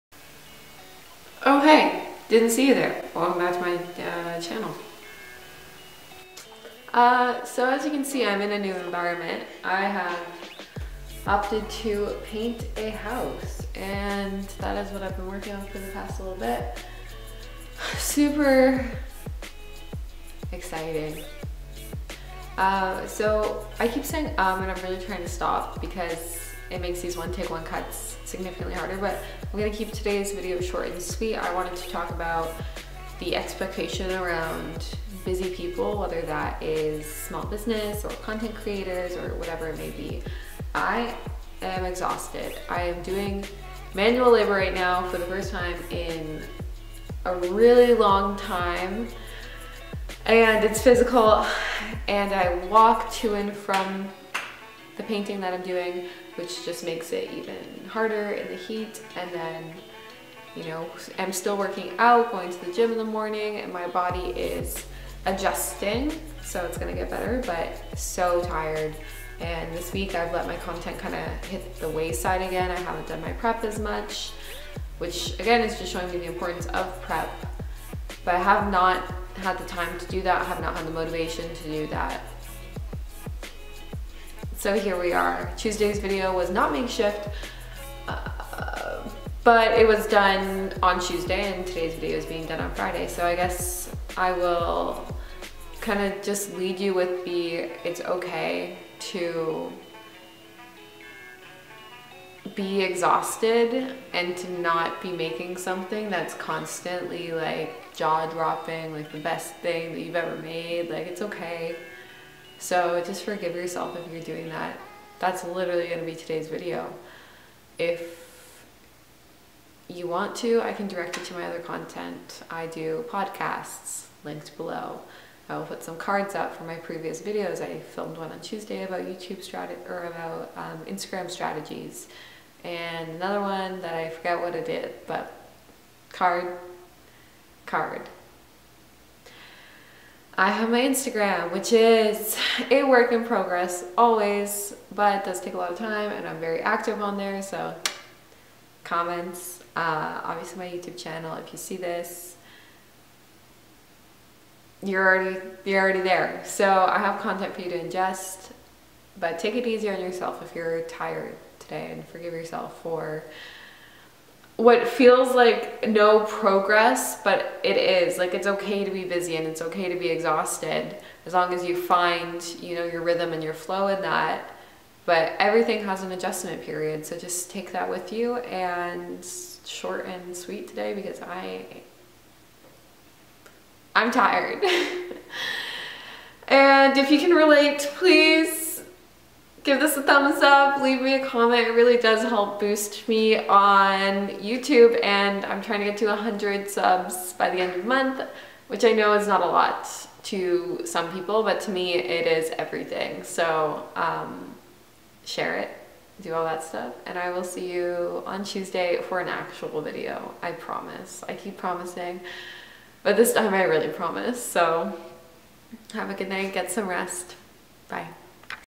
REAL TALK: I'm exhausted
A famous one take one cut. Don't feel bad for taking breaks, burnout is not fun :)